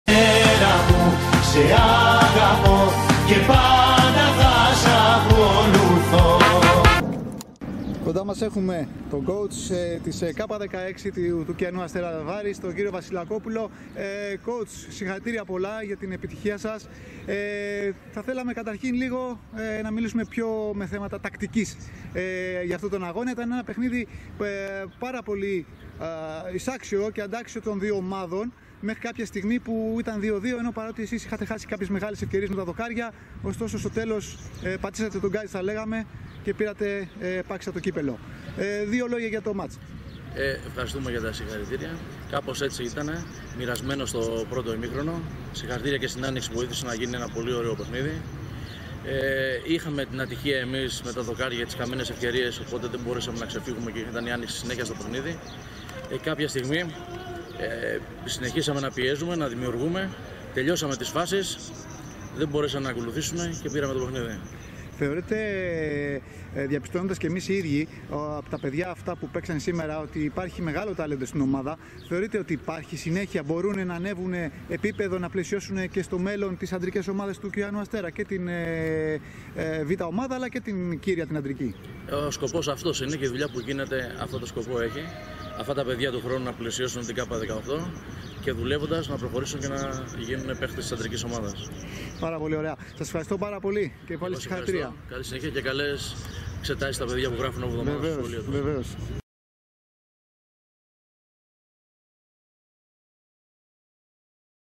Κυπελλούχος ΕΠΣΑΝΑ K16: ΚΥΑΝΟΥΣ ΑΣΤΗΡ ΒΑΡΗΣ - Δήλωση Βασιλακόπουλος Παντελής (Προπονητής Κ16)
Κυπελλούχος ΕΠΣΑΝΑ K16: ΚΥΑΝΟΥΣ ΑΣΤΗΡ ΒΑΡΗΣ
Δήλωση Βασιλακόπουλος Παντελής
(Προπονητής Κ16 ΚΥΑΝΟΥΣ ΑΣΤΗΡ ΒΑΡΗΣ)

📷   Γιώργος Τσιφλίκας   📹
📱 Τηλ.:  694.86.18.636